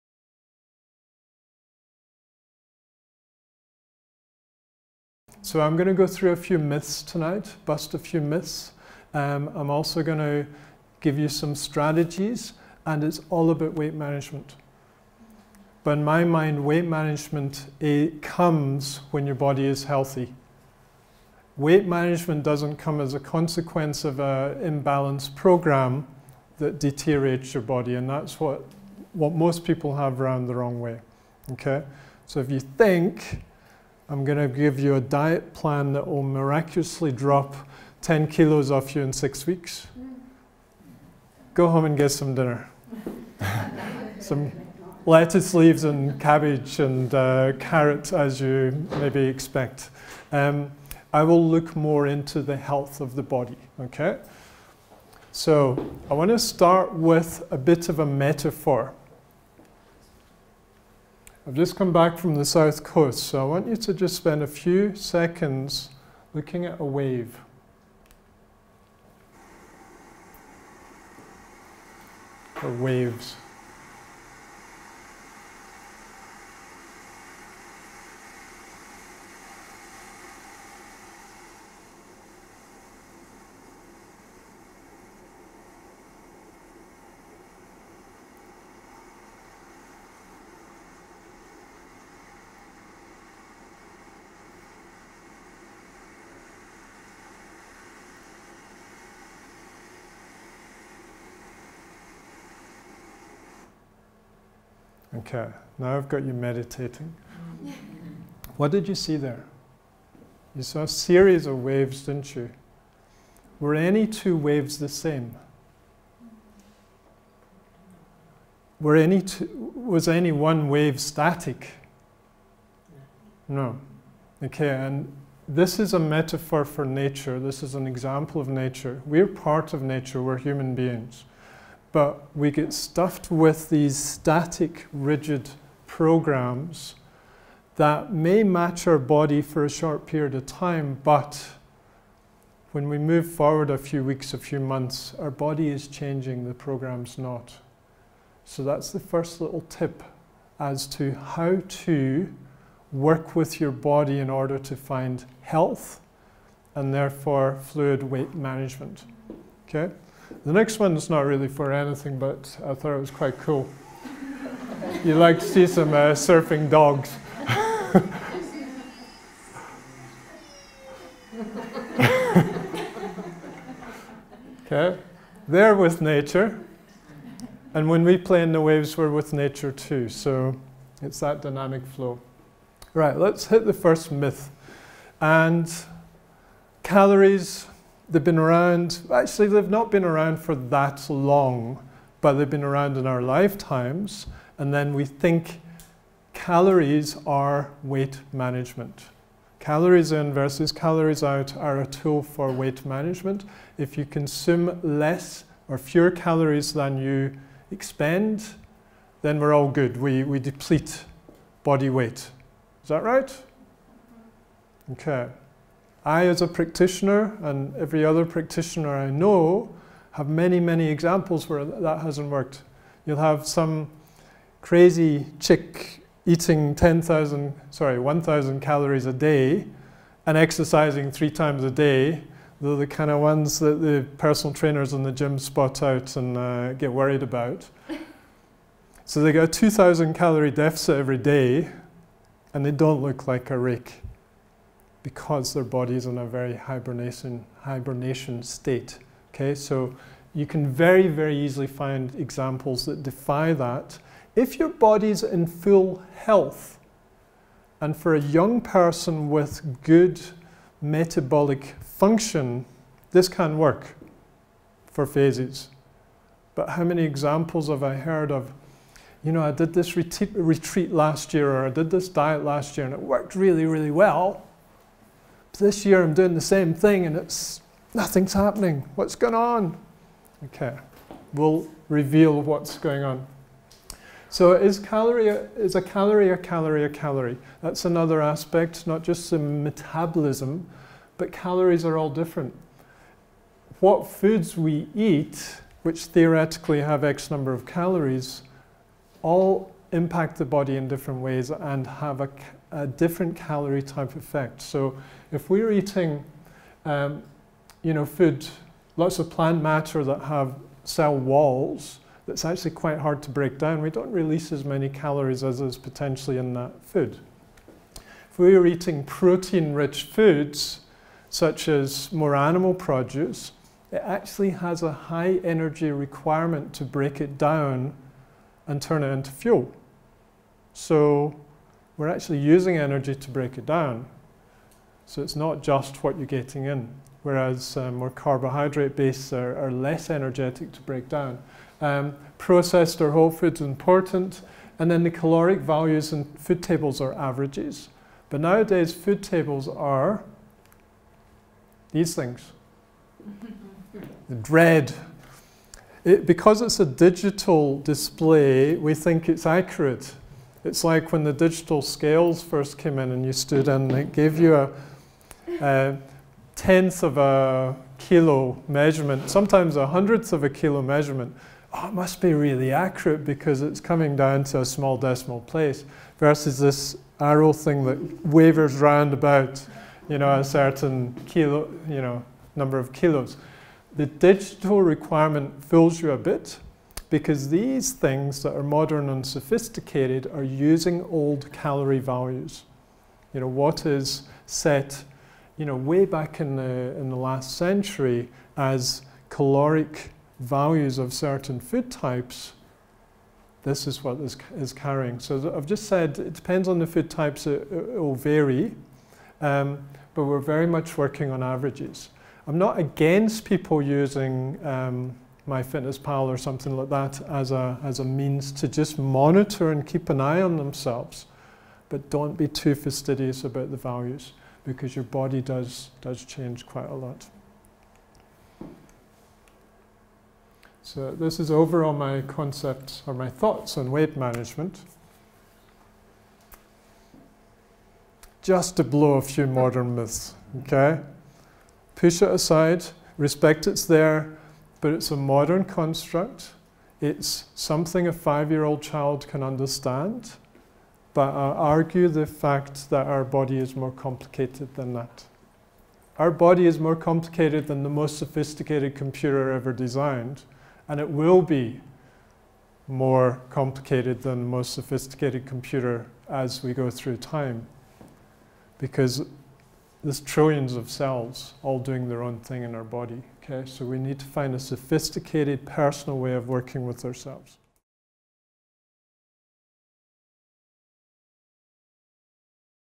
Shape up for Summer presentation - Ian Craig
In conjunction with Jeanne van Zyl, Ian gave a presentation on weight management from a nutrition, exercise and lifestyle perspective. He bust a few myths such as the calorie-in vs. calorie-out story and high-fat or low-fat for weight management. He then presented his top-6 weight management tips.